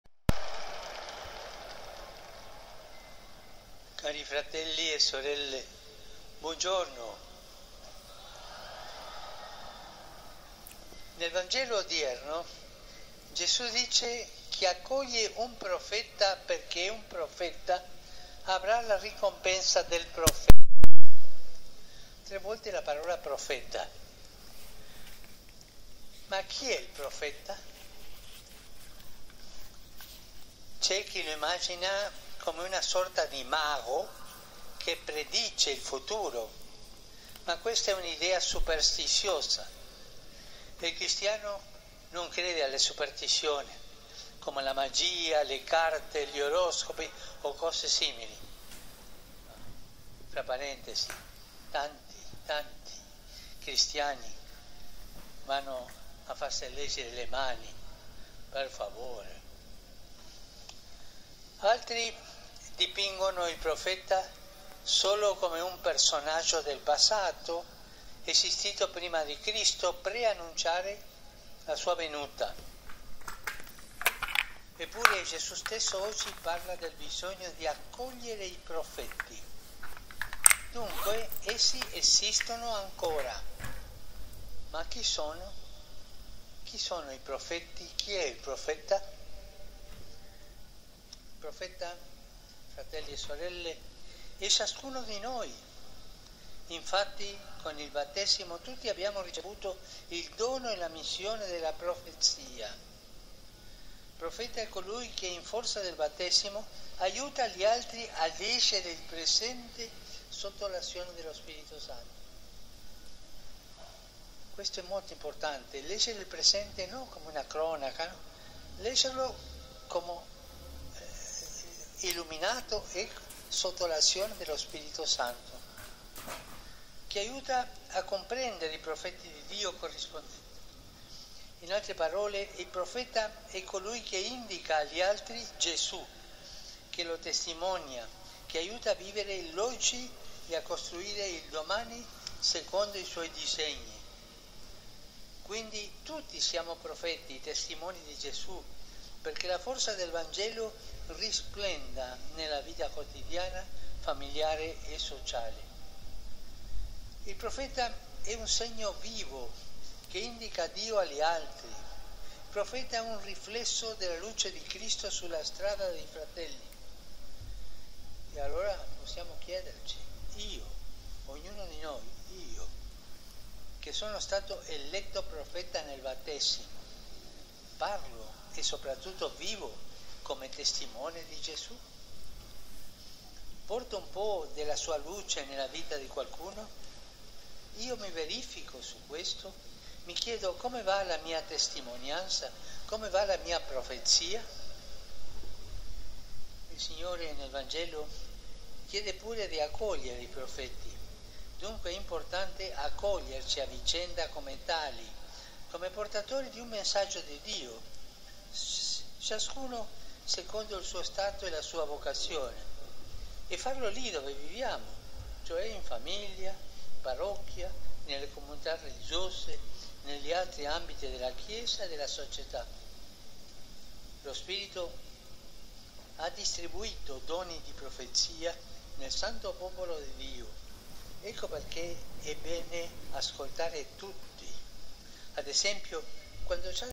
Angelus 230602 2023 July 2, soon with subtitles and transcript.
At Catholic Mass,  July 4 4, 2023, Matthew 10,37-42 is read.

I share asap this video with subtitles and a transcript that can also be easily translated into any language with a translation extension.

Pope Francis focused on who is a prophet. He says that anyone of us spreading and sharing the words of Jesus is a Prophet.

I am very happy that the Vatican youtube channel shares the Angelus with Italian Sign Language (ISL)

It may however not be understood by people in other countries. So I continue to share subtitles and transcript.

I wish you a good Sunday